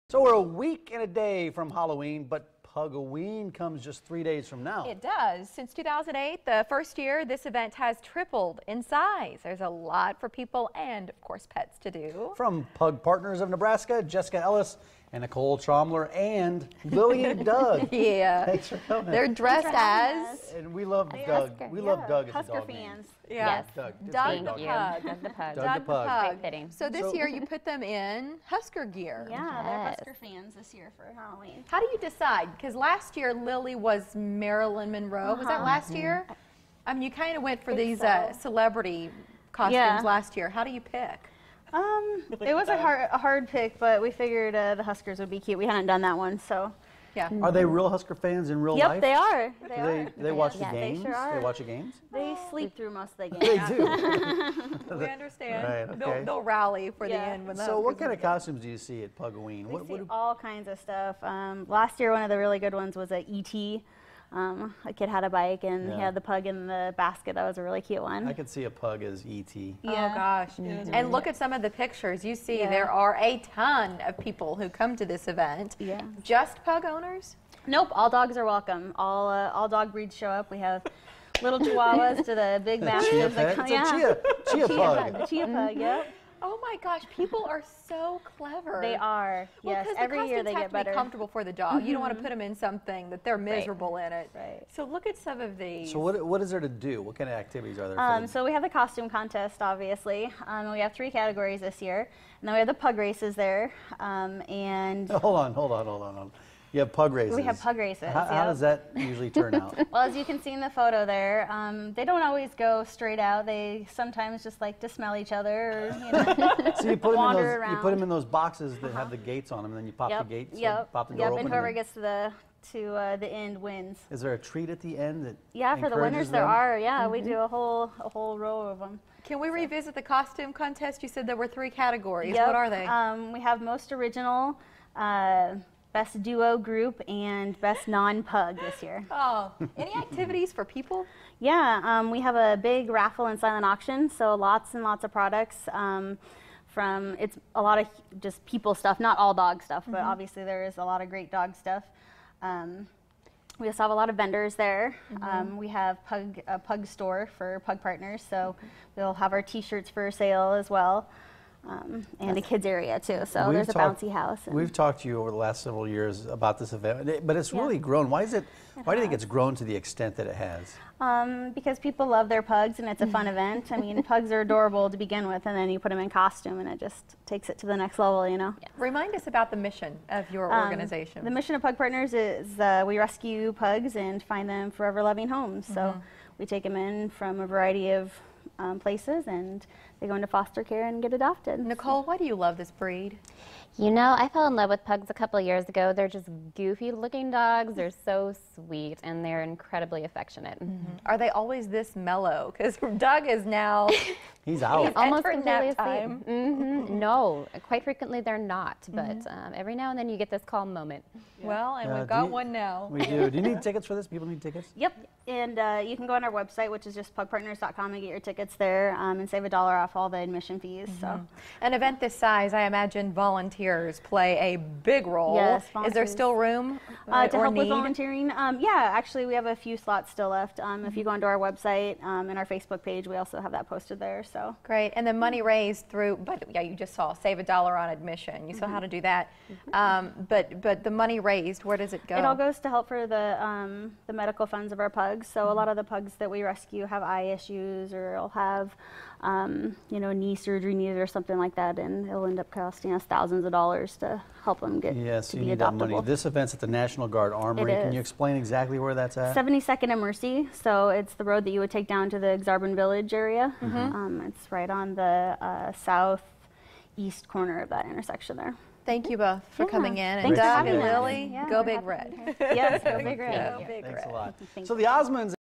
Pug Partners of Nebraska
It is time for Pug Partners of Nebraska to host their Annual Pug-O-Ween! On Sunday October 26, 2014 Pug Partners of Nebraska will be hosting their 6th Annual Event! Several dog friendly activities await guests, including contests! This event will take place from 12-4 PM at the National Guard Armory in Omaha.